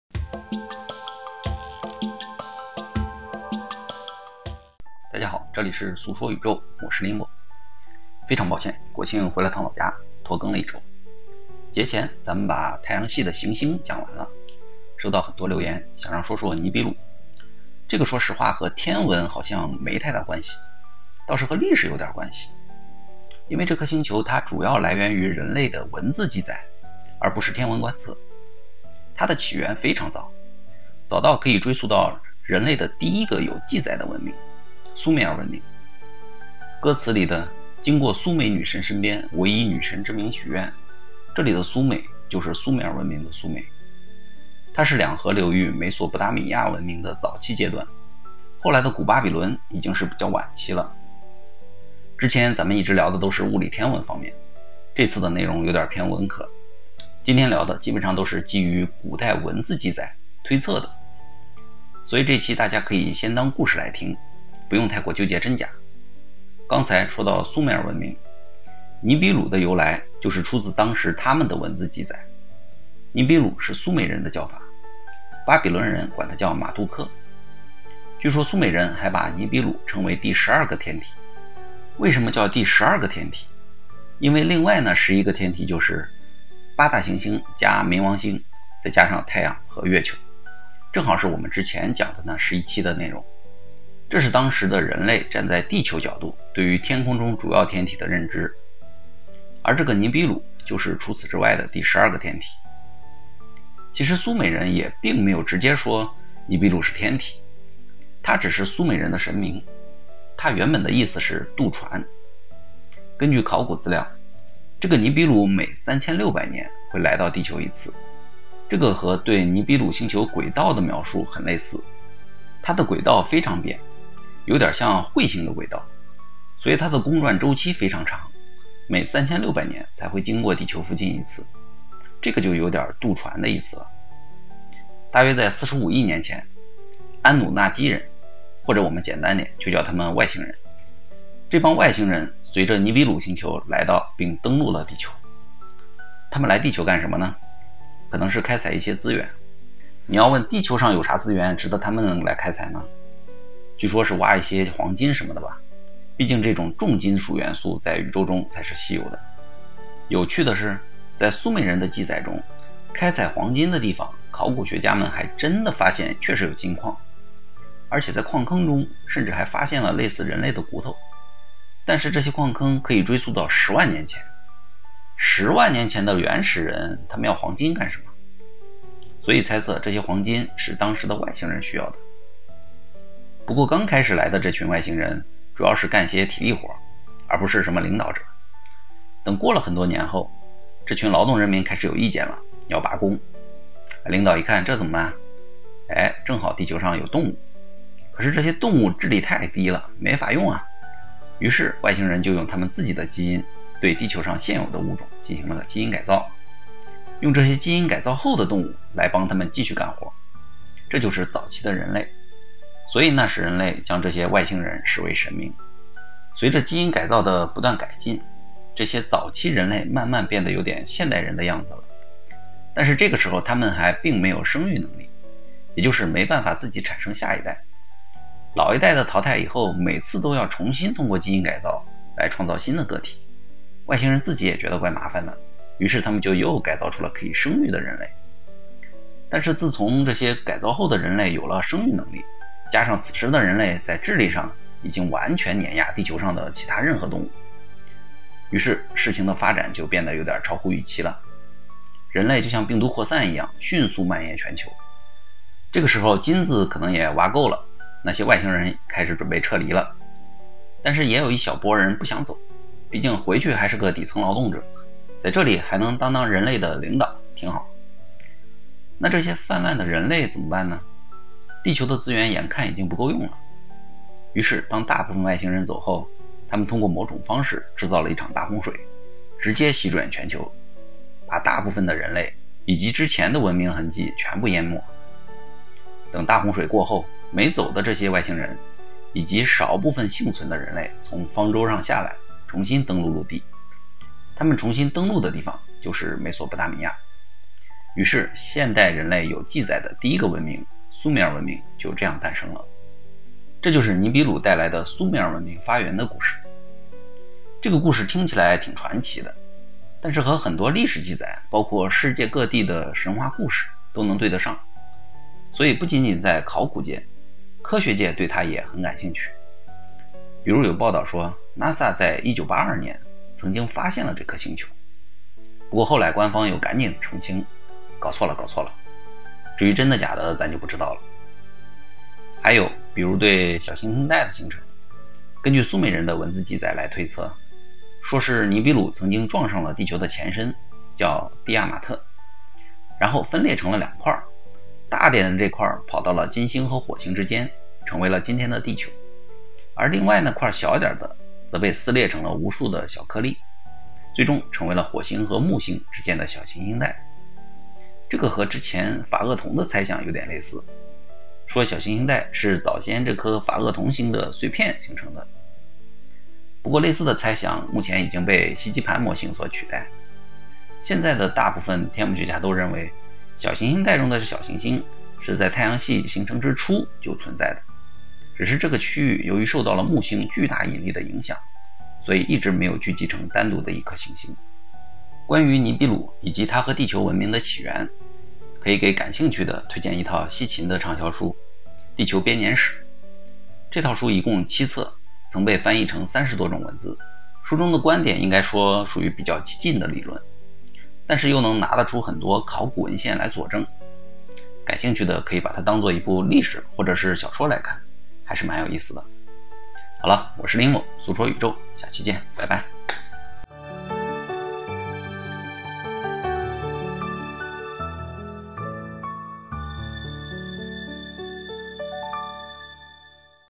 【俗說宇宙】第十大行星 尼比魯 ？人類的起源？
本期非科普範疇，請當故事來聽:)